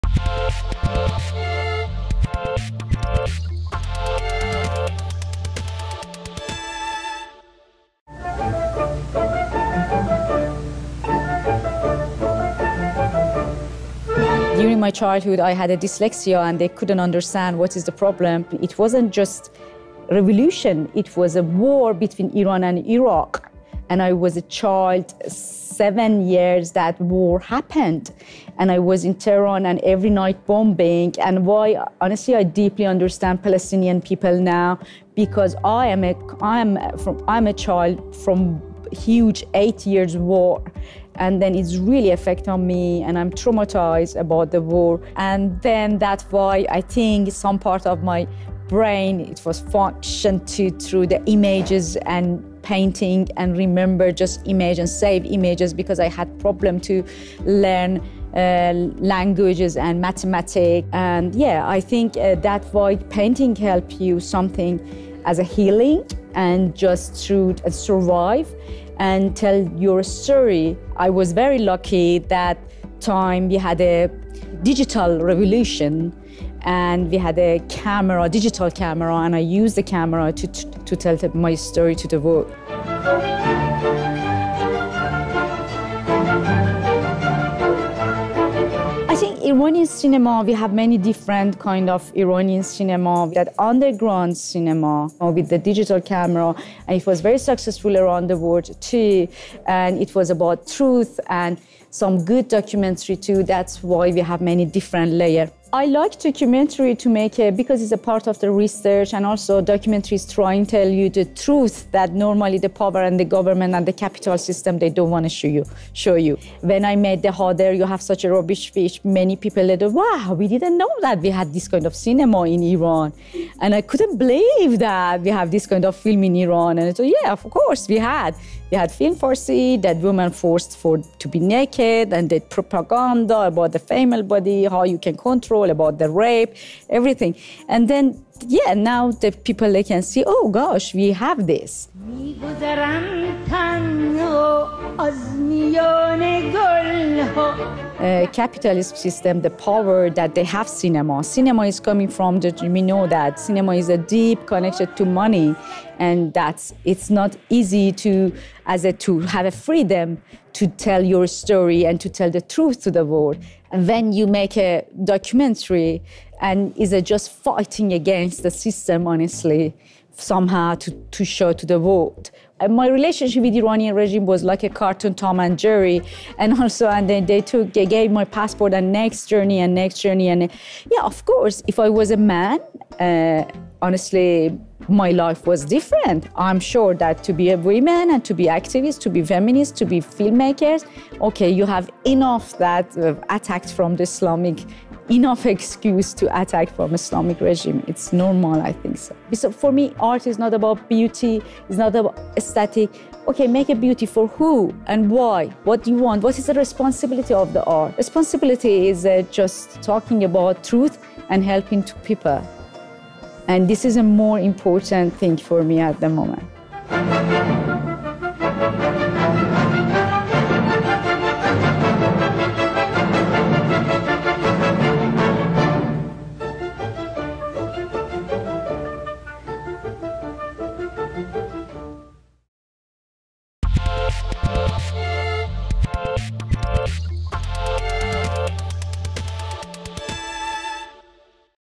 DOCUMENTÁRIO DISCUTE A IMAGEM DA MULHER NO IRÃ
O longa “Como Você se Atreve a Desejar Algo tão Terrível”, da cineasta iraniana Mania Akbari, costura trechos de filmes do Irã para fomentar uma reflexão sobre a figura feminina no país.